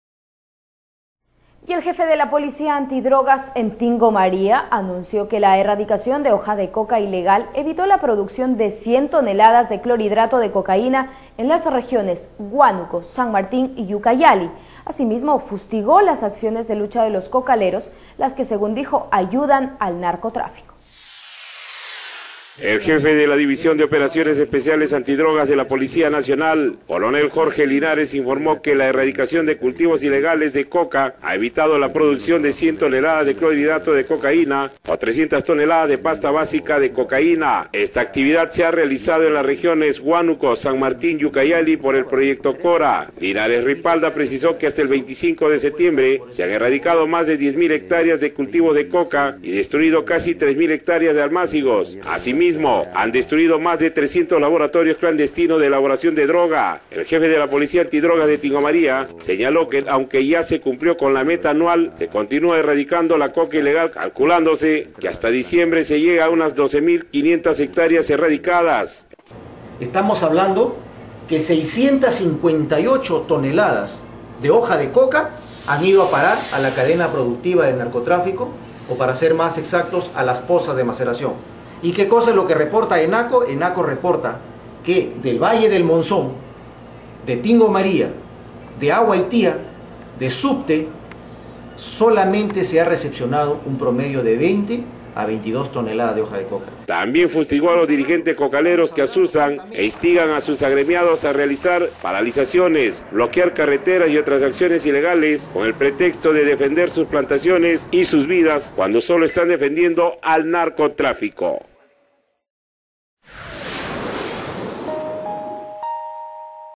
Erradicación de hoja de coca impide producción de 100 toneladas de clorhidrato de cocaína
El jefe de la División de Operaciones Especiales Antidrogas de la Policía Nacional, coronel Jorge Linares, informó que la erradicación de cultivos ilegales de coca ha evitado la producción de 100 toneladas de clorhidrato de cocaína o 300 toneladas de pasta básica de cocaína. Esta actividad se ha realizada en las regiones Huánuco, San Martín y Ucayali por el proyecto Cora.

Linares Ripalda precisó que hasta el 25 de setiembre se han erradicado más de 10 mil hectáreas de cultivos de coca y destruido casi tres mil hectáreas de almácigos. Asimismo han destruido más de 300 laboratorios clandestinos de elaboración de droga.

El jefe de la Policía Antidrogas de Tingo María, señaló que aunque ya se cumplió con la meta anual se continúa erradicando la coca ilegal calculándose que hasta diciembre se llegue a unas 12 mil 500 hectáreas erradicadas.

El oficial indicó que aproximadamente 658 toneladas de hoja de coca cultivadas en la zona han ido al narcotráfico, en tanto que Enaco acopió sólo 22 toneladas.